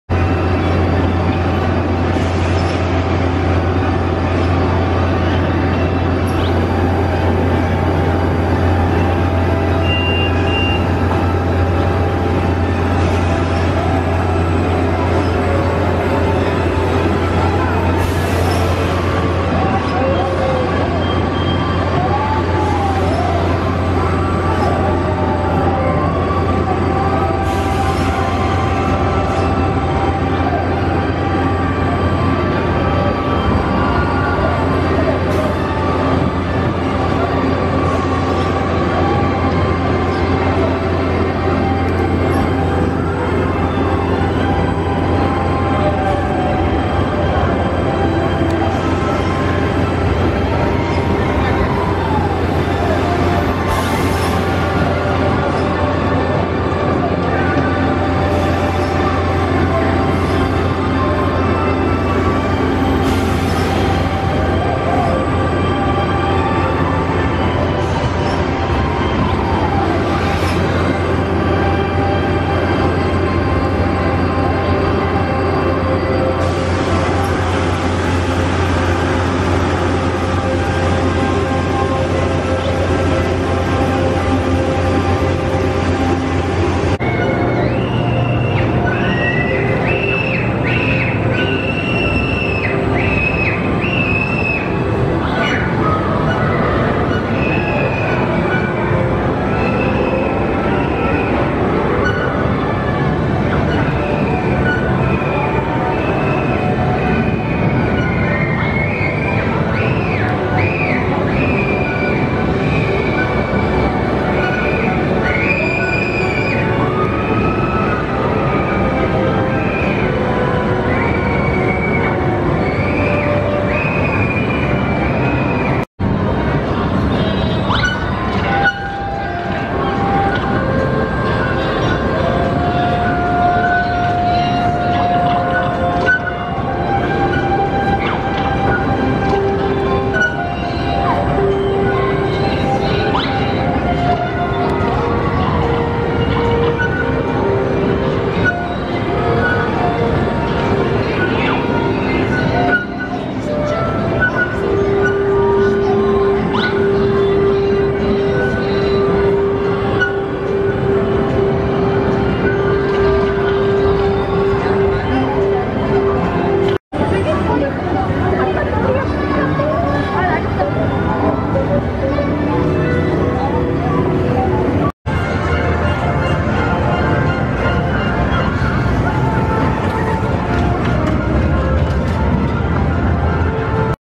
Screamin' Centipede Off-Ride Footage, Tom Foolerys Adventure Park Big Air Coaster | Non-Copyright
Screamin' Centipede at Tom Foolerys Adventure Park just recently opened last month.  This SBF Visa Big Air Coaster combines spinning cars with a hamster wheel car to offer both a family-friendly and thrilling ride experience.  Check out the signature coaster at the newest Kalahari Resort in Round Rock, Texas!